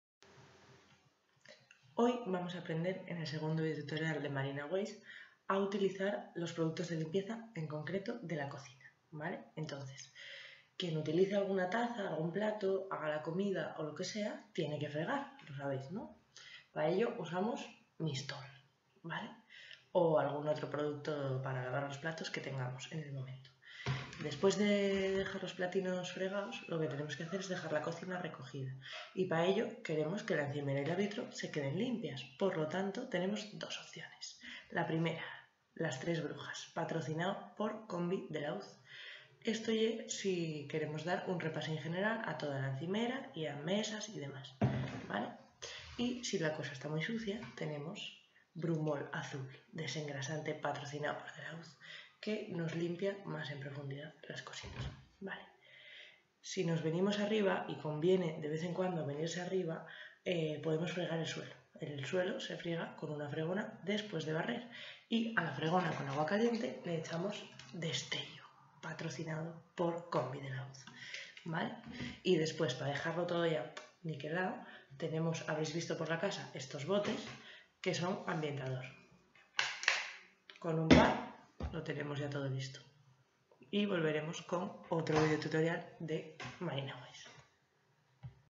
Como usar una freehouse - Limpiando la cocina con productos delaUz
En este capítulo veremos los productos de limpieza de de la uz, aplicados a tener la cocina super limpia